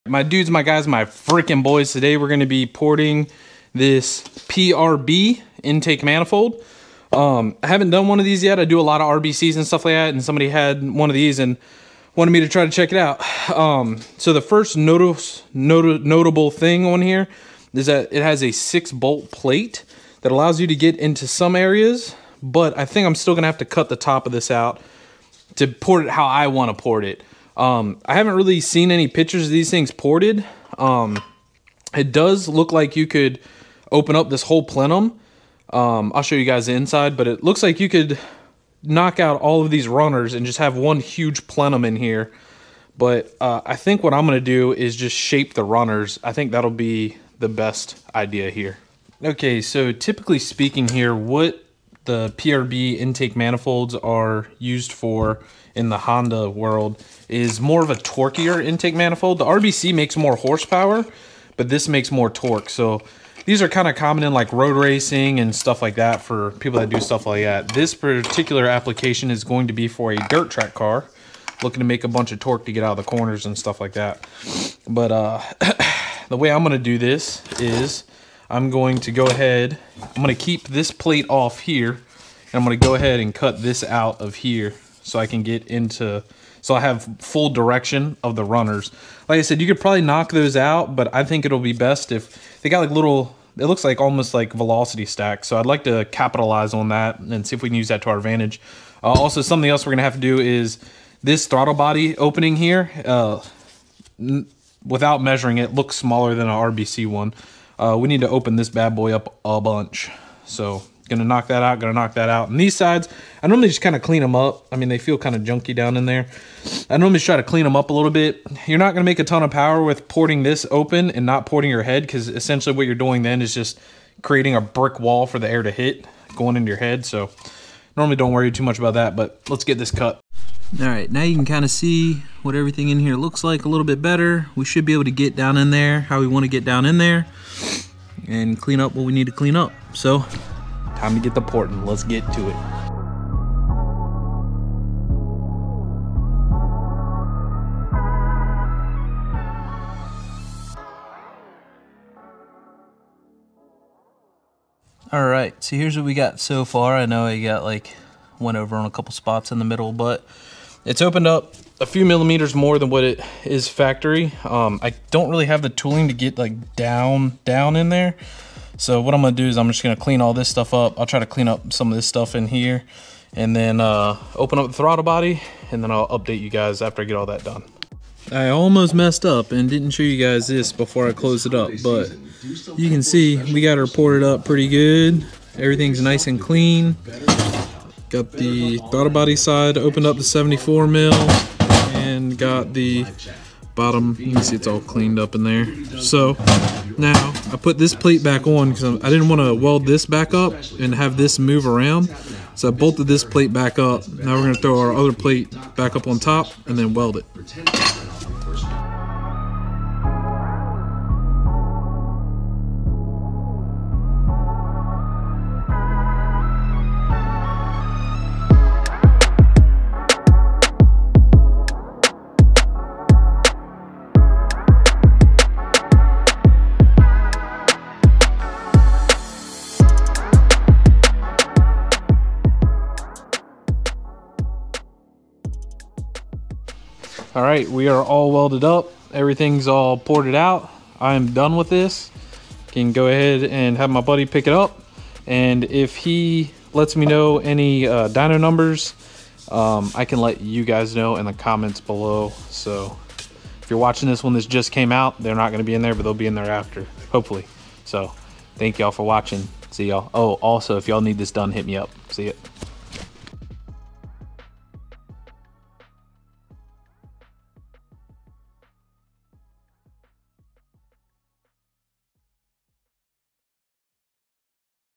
PRB, The RBCs Little Brother? Lets Port It!
Im curious to see how this performs on this application. We have tested stock PRB manifolds on k24s and have seen a noticeable difference in torque gains. Porting one like this is a interesting test, id also like to just knock out all the material and just have a huge plenum and see how that does!